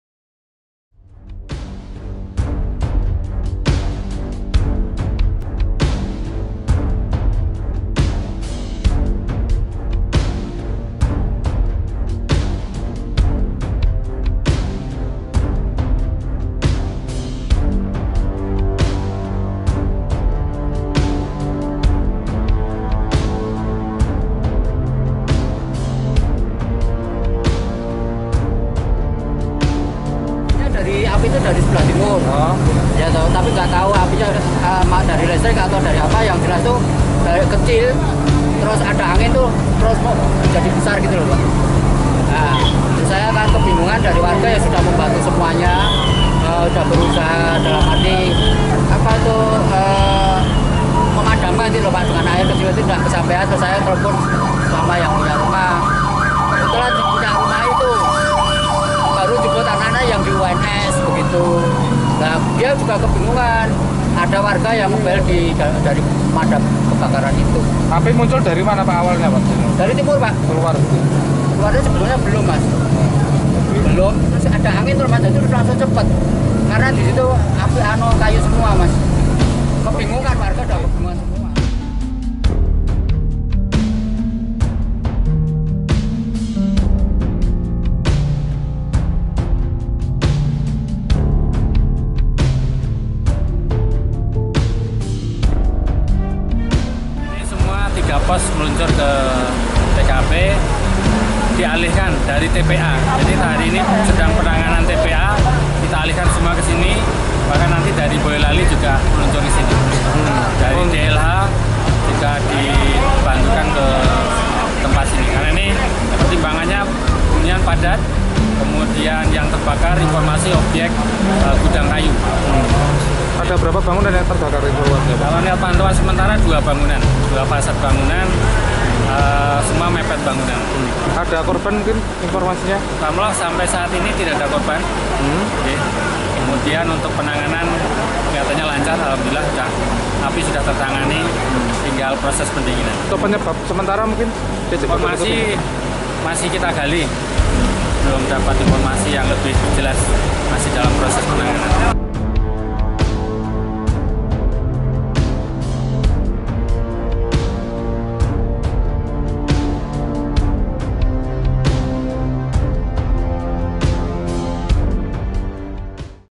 Kebakaran,Gudang Kayu Dan Satu Rumah Ludes
Kebakaran Gudang Kayu Di Banyuagung,Banjarsari,Solo, Sempat Menghebohkan Sejumlah Warga Yang Ada Sekitar Lokasi. Pasalnya Api Dengan Cepat Membakar Dua Bangunan, Yakni Rumah Dan Gudang Kayu. Beruntung Dalam Peristiwa Ini Tak Ada Korban Jiwa, Api Berhasil Dipadamkan, Setelah Petugas Pemadam Kebakaran Tiba Di Lokasi Kejadian.